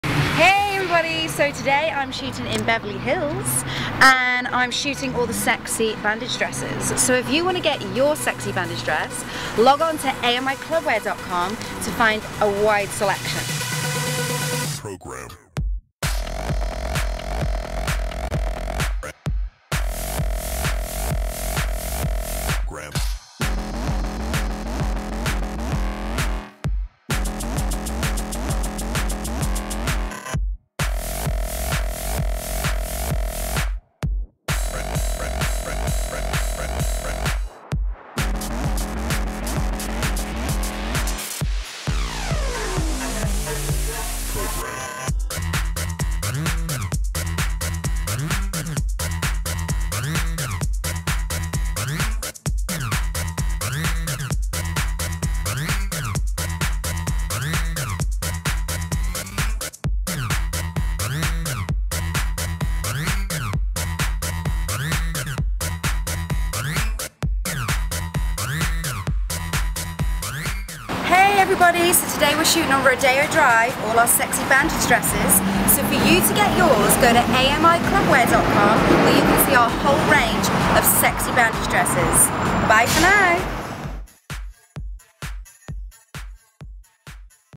JAMIE LEE Goes to Beverly Hills to Show Off Our Sexy Bandage Dresses
Jamie Lee does a photo shoot in Beverly Hills.